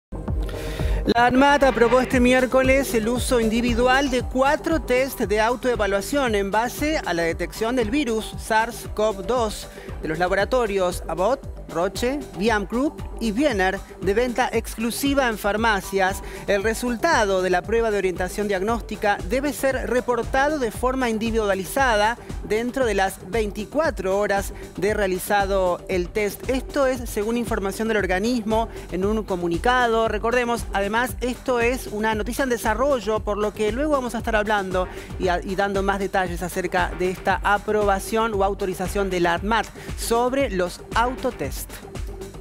ANMAT autorizó los autotests de Covid-19
La Administración Nacional de Medicamentos, Alimentos y Tecnología Médica (Anmat) autorizó este miércoles la utilización de cuatro test de autodiagnóstico de Covid-19 que podrán ser adquiridos en farmacias. Los insumos corresponden a los laboratorios Abbott, Roche, Vyam Group y Wiener.